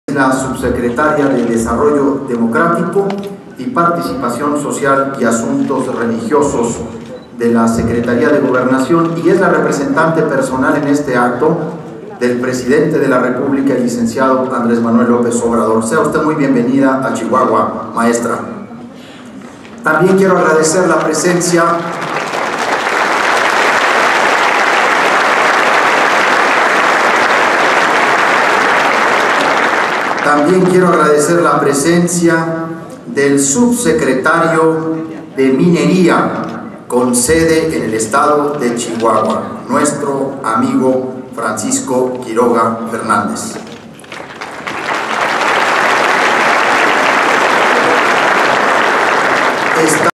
En 2018, rehabilitaron 46 Casas de Cuidado Diario, 58 Centros de Bienestar Infantil: Javier Corral
El Gobernador señaló: Estamos comprometidos con ser el estado que más y mejor cuida a sus niñas y a sus niños.
Así, durante 2018 año se rehabilitaron 46 Casas de Cuidado Diario, 58 Centros de Bienestar Infantil y tres Centros de Atención, Cuidado y Formación Infantil en Ciudad Juárez, con una inversión de 28 millones de pesos. Con esto rehabilitamos o creamos 1 mil 246  espacios para cuidado infantil.
Además, invertimos 25.3 millones de pesos para becar a cinco mil 403 niñas y niños en diferentes modalidades de cuidado, mucho más que cualquier otro gobierno anterior.
Queremos anunciar que a las estancias infantiles que por ahora no se ha resuelto el apoyo o la forma en que se realizara el apoyo del  Gobierno Federal no las vamos a abandonar. Estamos, desde ahora, procurando los recursos y buscando formas de asociación con diferentes municipios para que sigan funcionando, incluso de mejor manera. Ninguno de los niños que acuden a ellas se quedará sin cuidado.